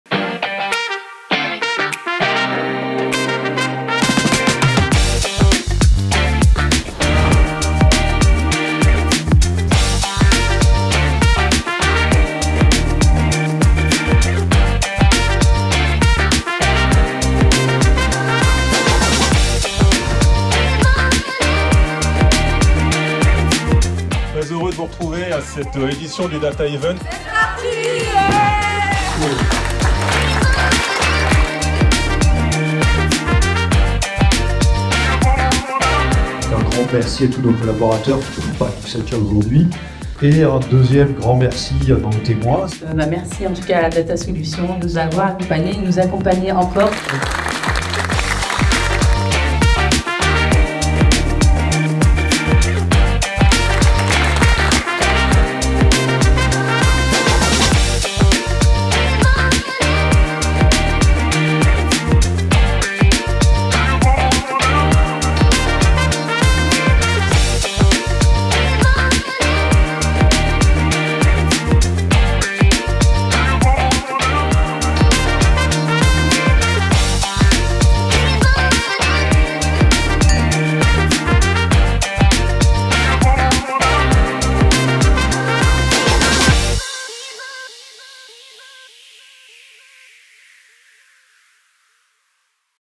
Retour sur le DATA EVENT 2024
Les meilleurs moments du DATA EVENT 2024, notre évènement client annuel organisé le 3 octobre 2024 à la Maison A Trocadéro.
Une après-midi d'échanges et de retours d'expérience avec nos clients et partenaires, suivie d'un cocktail.